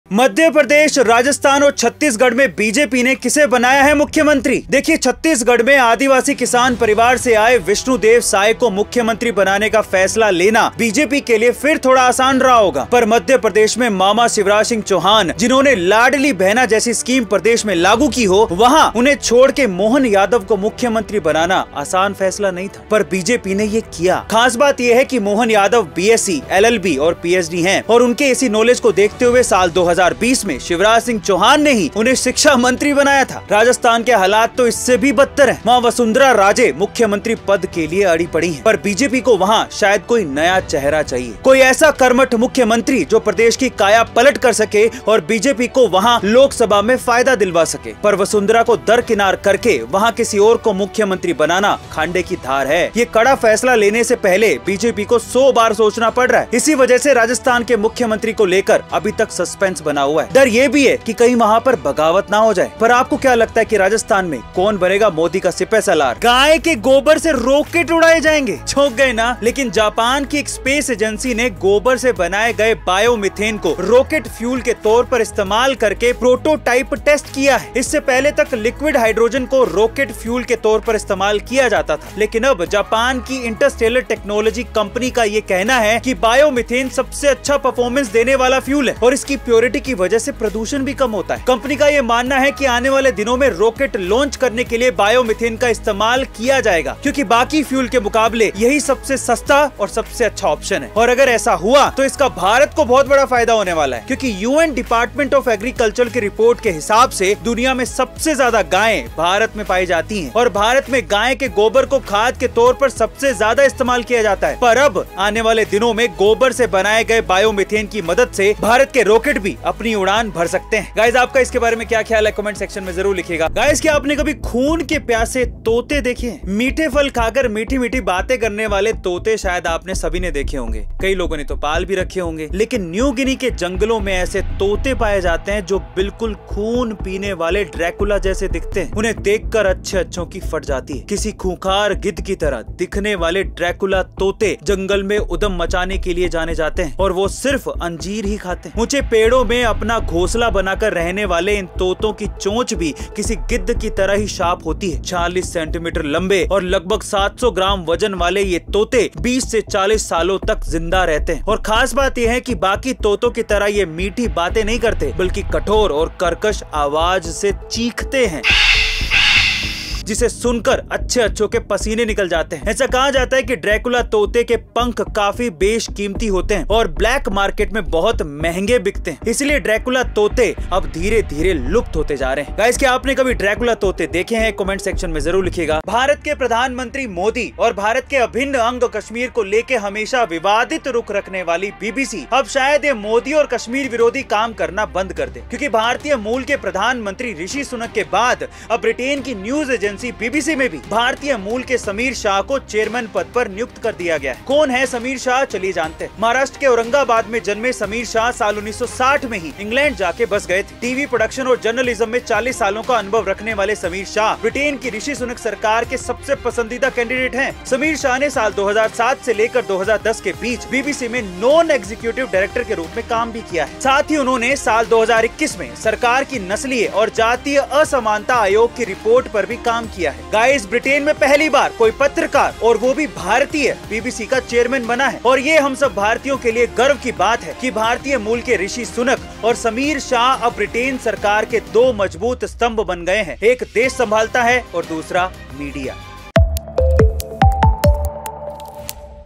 राजस्थान , MP छत्तीसगढ़ में कौन बना मुख्यमंत्री ? mukhyamantri youtubevideo by arvindarora
Following are some of the amazing facts discussed in this video:
1. Who became the Chief Minister of Rajasthan, MP Chhattisgarh?
2. Rocket will fly with cow dung 😳
3. A parrot who is thirsty for blood 😳
4. Why won't BBC do anti-Modi/Kashmir work now?

इस वीडियो में चर्चा किए गए कुछ आश्चर्यजनक तथ्य निम्नलिखित हैं:
1. राजस्थान , MP छत्तीसगढ़ में कौन बना मुख्यमंत्री ?
2. गाय के गोबर स उड़ेगा रॉकेट 😳
3. ऐसा तोता जो है खून का प्यासा 😳
4. मोदी / कश्मीर विरोधी काम अब क्यों नहीं करेगा BBC ?